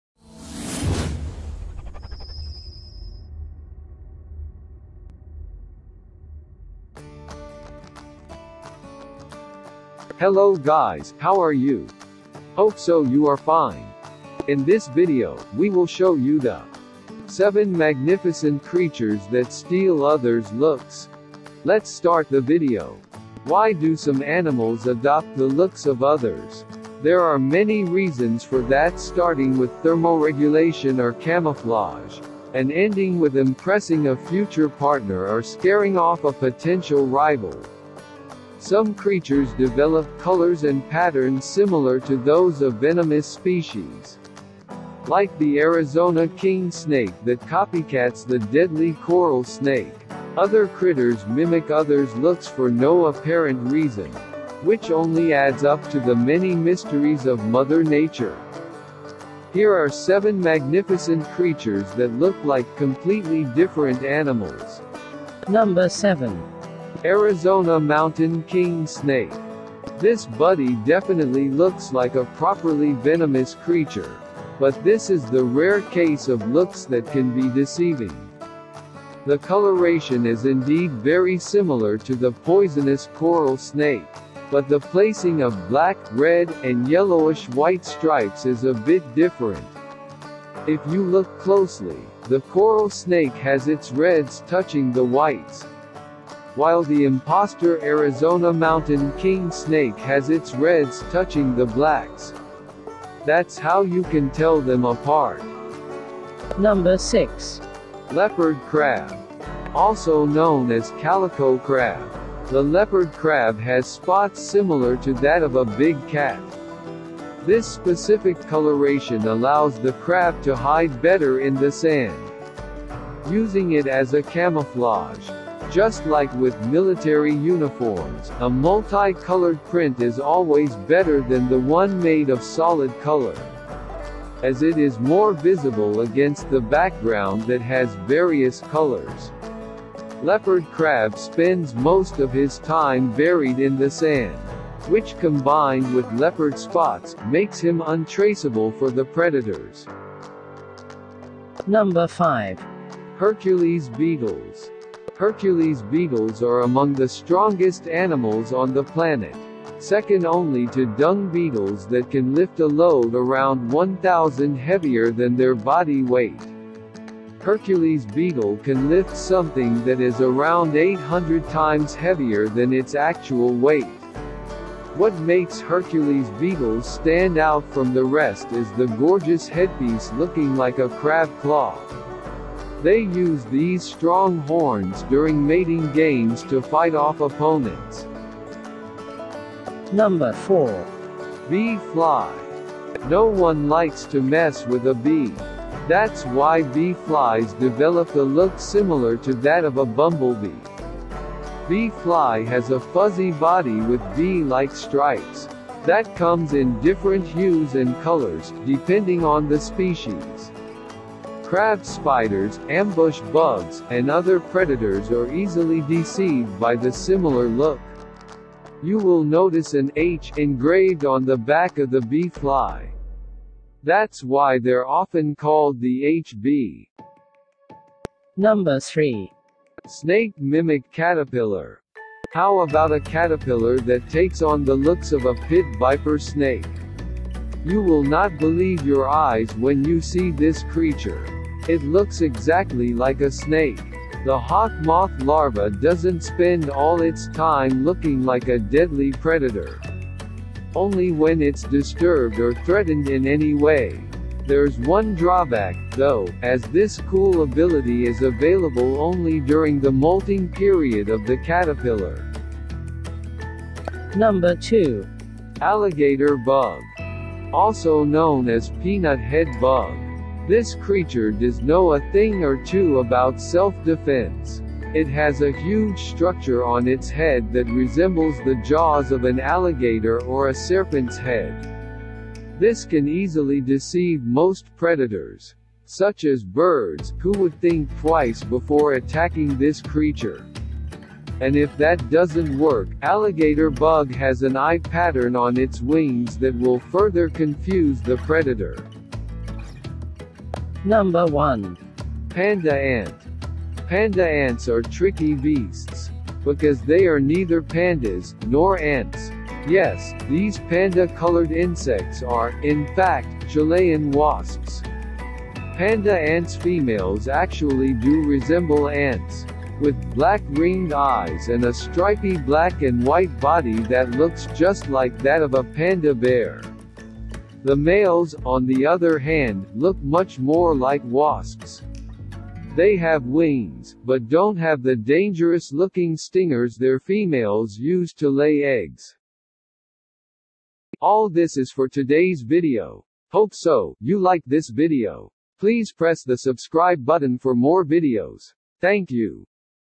7 Magnificent Creatures That Steal Others’ Looks | amazing creatures of allah
Hey guys
7 magnificent creatures that steal other's look
About this video:
In this video we will show you top awesome creatures of God, present in this world. We collect these creatures with utmost struggle.
Hope so you like this video